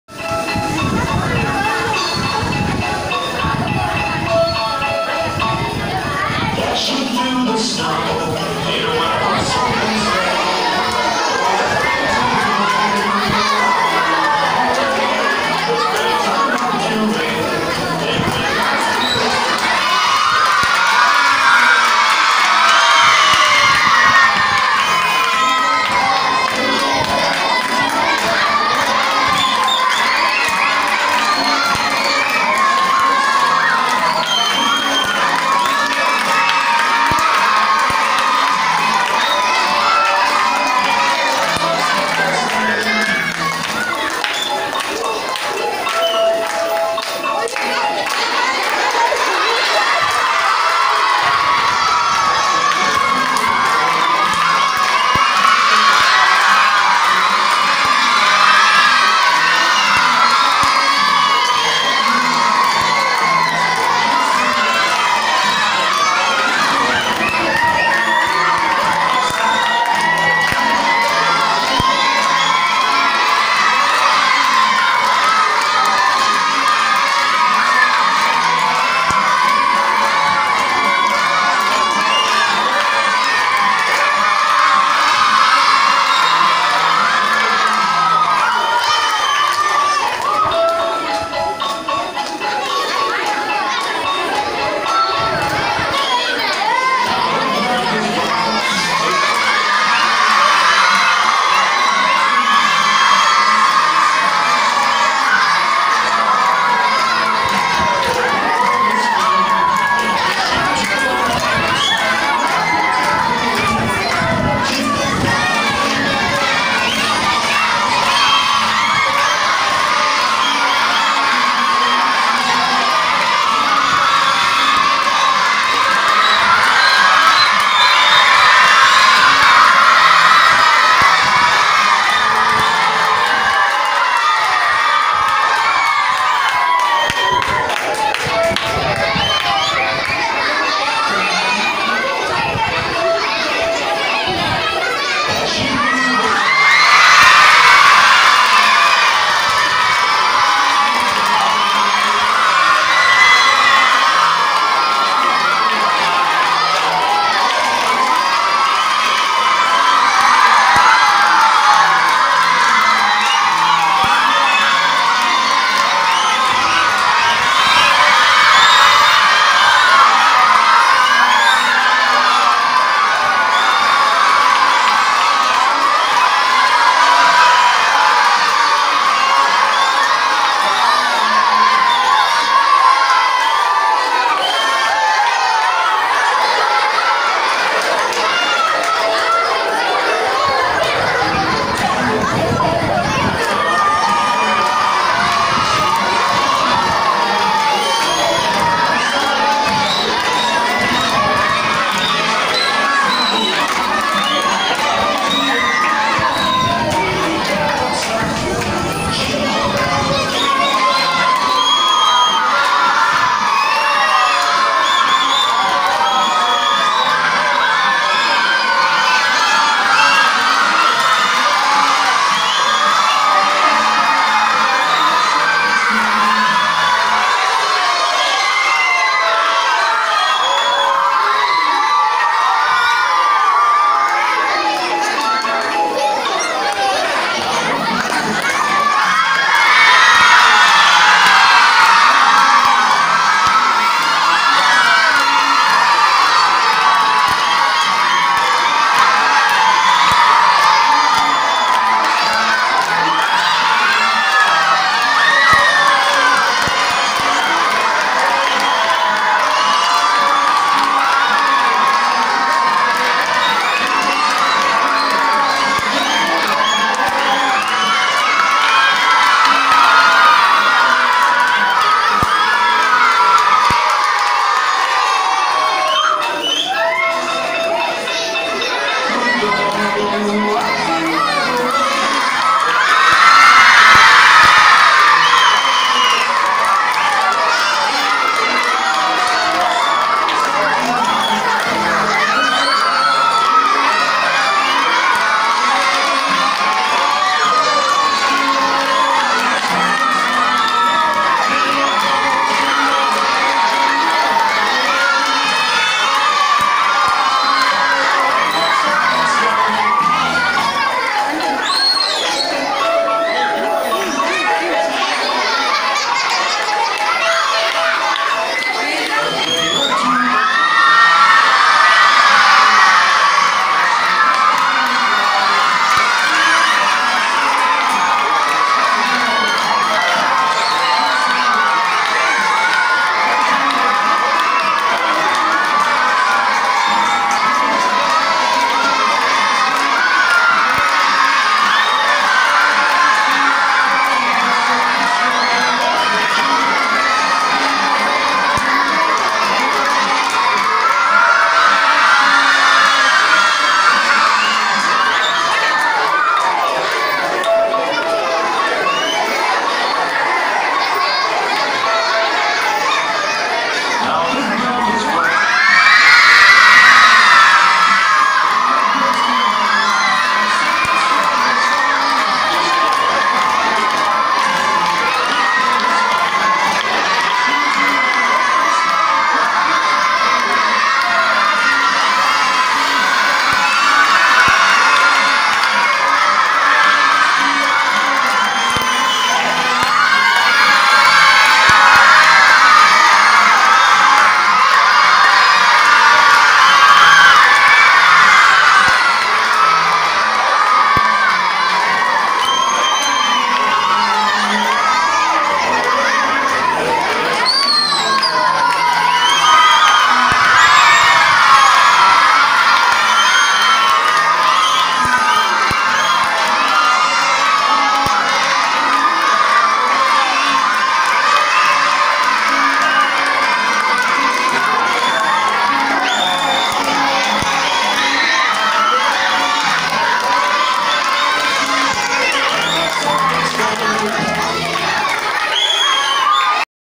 Aacini Matric Higher Sec School Ambattur #🎄 🎄🎅🎅Christmas Day celebration🎅🎅🎅🎅#
🎅🎅Christmas can bring entertaining, 🎄🎄distraction from the 📖📖study stress brought by the entire School year. 👭👬The Children feel excited, joyous. The School environment gets uplifted with the positive energy of delight and Celebration. Children Learn the Difference between cheer and Selflessness that bind Mankind together. 🎄🎄🎄🎅🎅🎅🎅🎅Children'sday# #Christmasday# bestdance# entertaining# positive# Kidscelebration#joyful# #